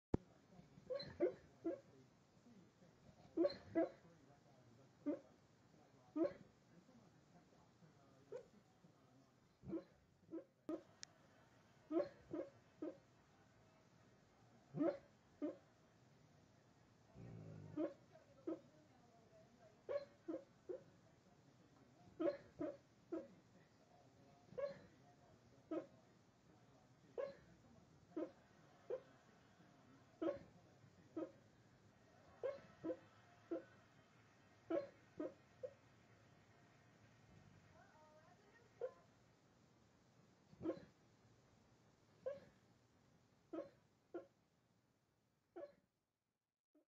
Wyatt barking in his sleep
The real funny part is the dog never barks when he's awake.  Only while he sleeps.  Not sure what thats about.